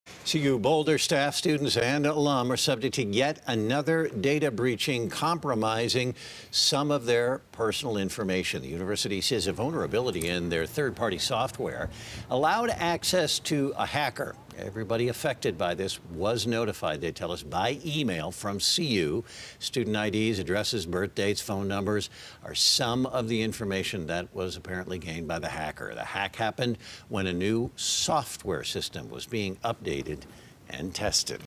Data Breach Hits University Of Colorado Boulder Campus
Another data breach compromised some personal information of staff, students and alum at the University of Colorado Boulder.